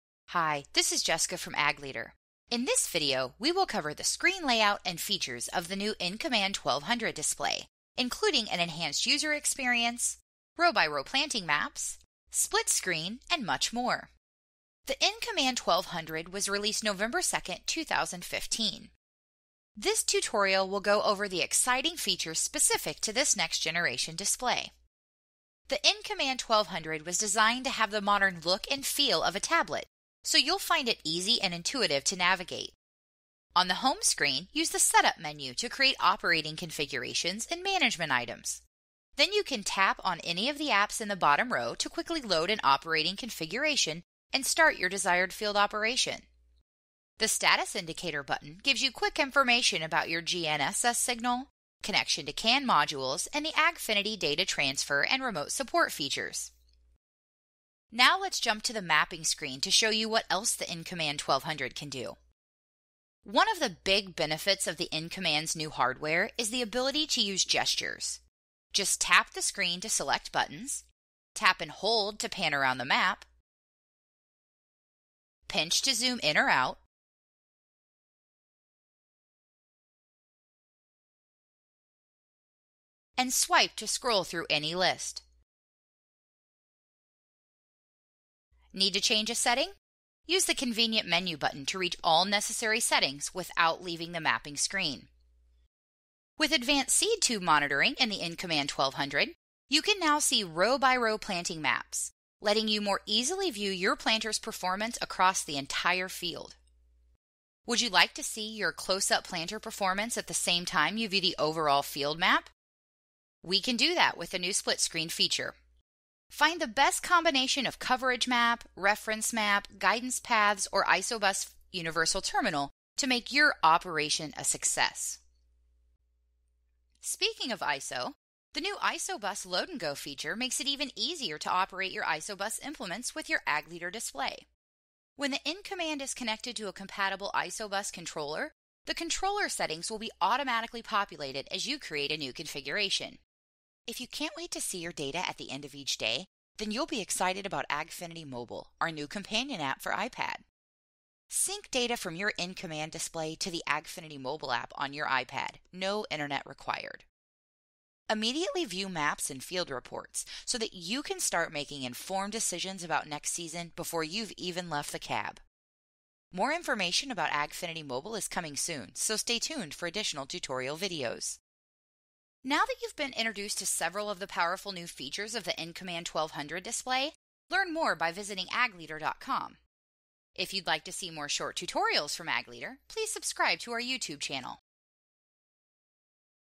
Welcome to InCommand – Feature Overview
Walkthrough of InCommand™ 1200 features, available at time of release in November 2015. Including Row x Row planting maps, Split Screen, and AgFiniti Mobile.